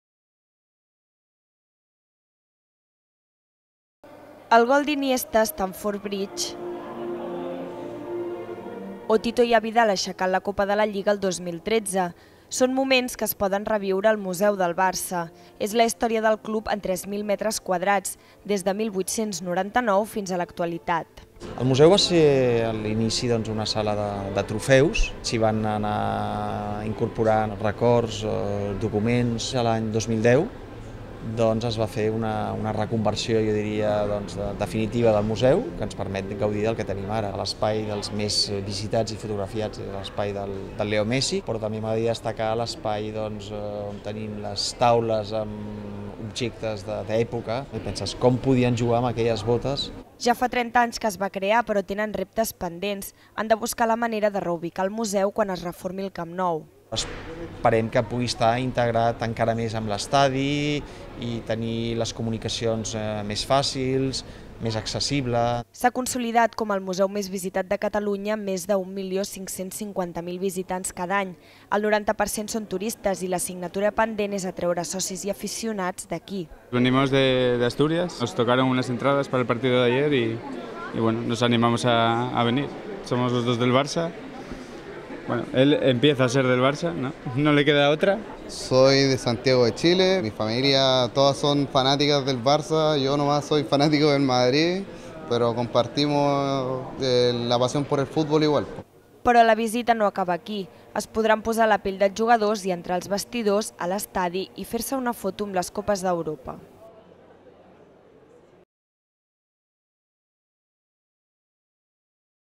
La història del Barça en 3.000 metres quadrats
El Museu del Barça ha celebrat 30 anys. És un espai de 3.000 metres quadrats que explica la història del Club amb pantalles interactives, documents, objectes i fotografies. A més, ofereix la possibilitat de visitar les instal·lacions de l'estadi i trepitjar, per exemple, la sala de premsa, la zona mixta o els vestuaris. Més informació